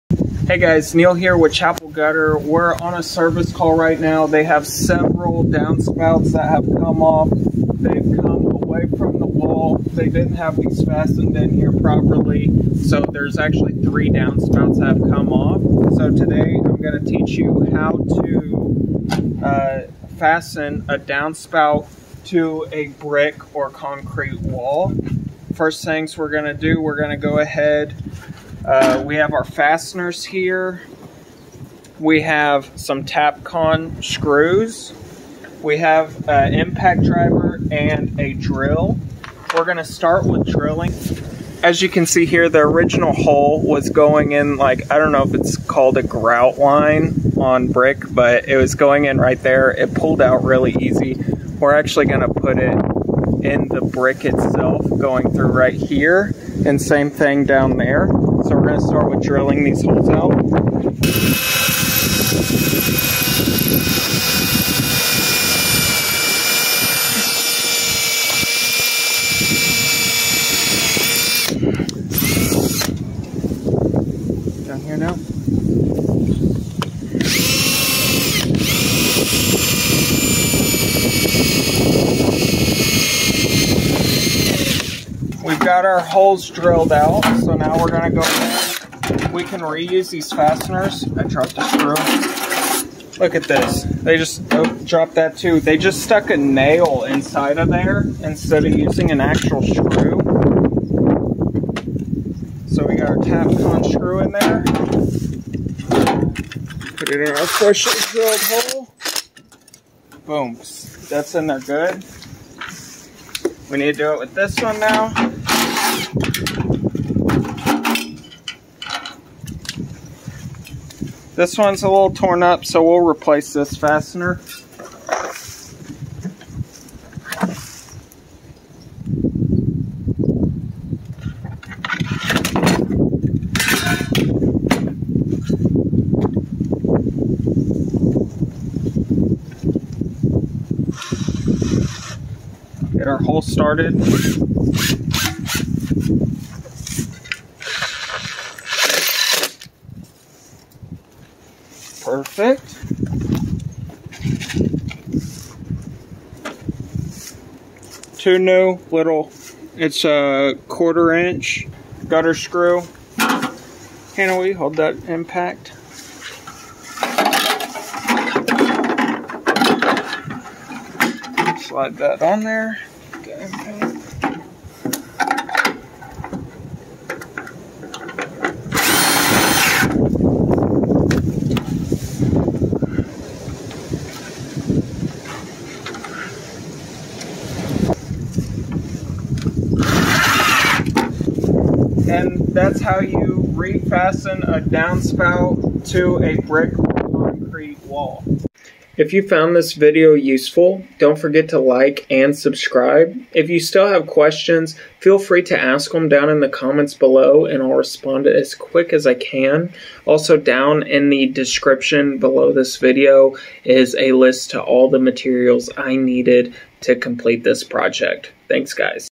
How To Refasten A Downspout Into Brick Siding | DIY Concrete Or Brick Downspout Installation
How to reinstall a downspout strap to brick or concrete DIY with the new Downspout Strap Repair Kit.

One of the most common issues we run into is downspouts coming detached from brick or stone siding.

It’s harder for bricks to hold a screw but it’s typically not the screw's fault for the downspout coming disconnected. In most cases, a gutter downspout will fall off because of improper downspout installation.

In this video, I’ll show you how to properly refasten or install a downspout to a home with brick or concrete siding.

*CHAPTERS:*
00:00 Introduction
00:25 Materials Needed
00:39 Drilling
01:26 Reinstall Straps
03:18 Outro

Drill & Impact Driver

3/16 in. Steel Carbide Tip Masonry Drill Bit

¼ in. Impact Duty Nut Driver

¼ in. guttering screws

Downspout Fasteners